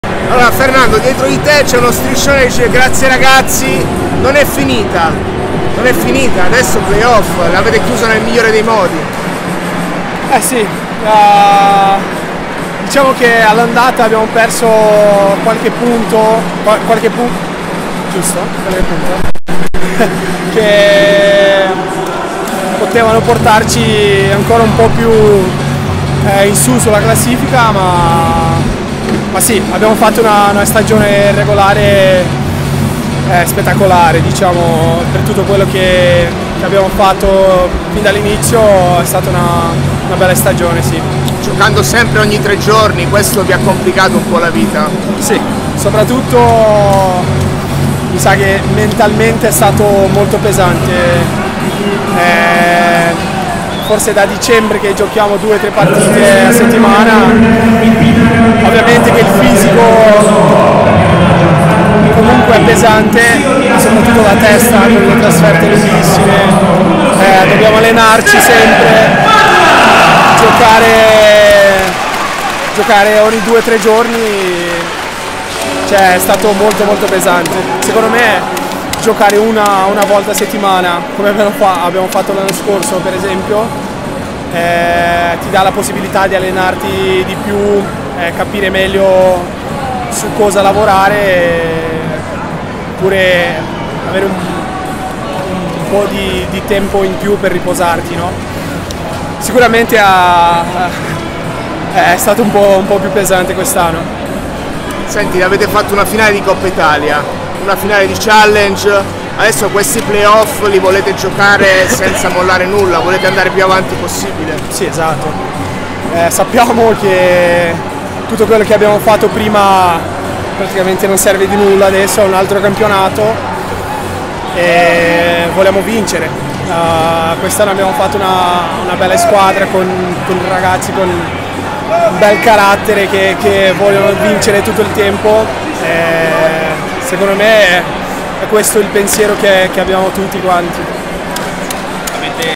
Cachopa: “Play Off? Questa è una squadra di ragazzi che vogliono vincere…”
Le parole di Cachopa dopo l’ultima giornata di regular season che ha visto Monza battere Costerna e chiudere al quarto posto

Intervista di Giuliano Bindoni